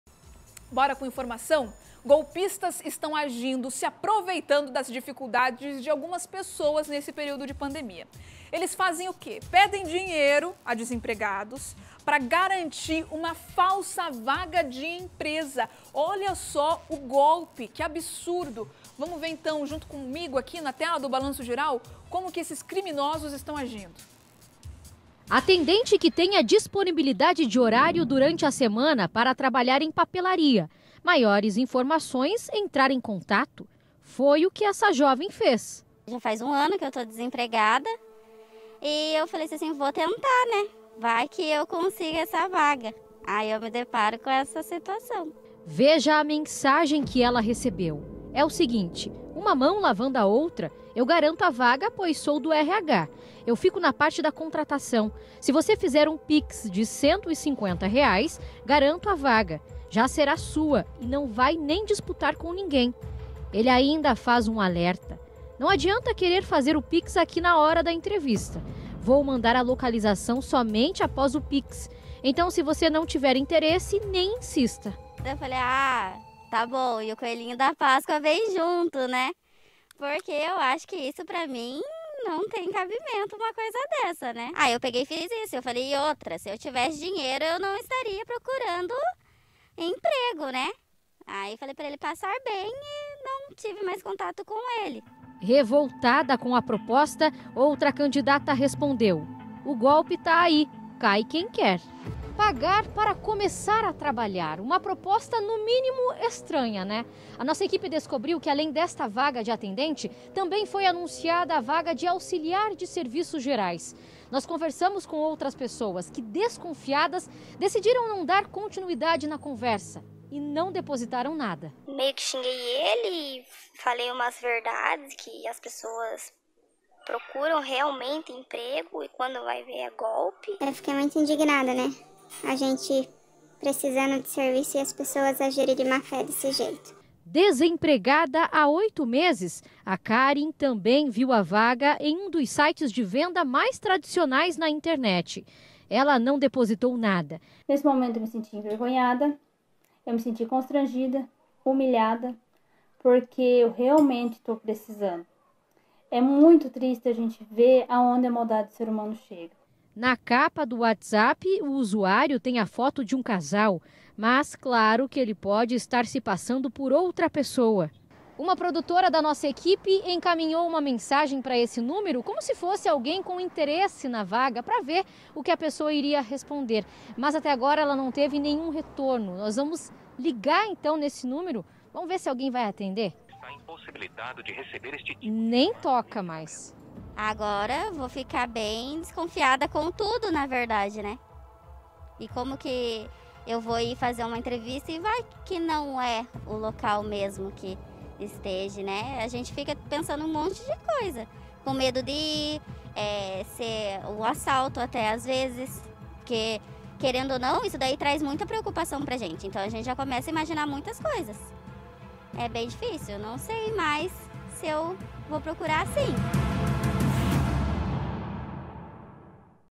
Bandido pede dinheiro para garantir falsa vaga de emprego em papelaria
Golpistas estão agindo, se aproveitando das dificuldades de algumas pessoas na pandemia. Eles pedem dinheiro a desempregados para garantir uma falsa vaga de emprego. Confira todos os detalhes na reportagem!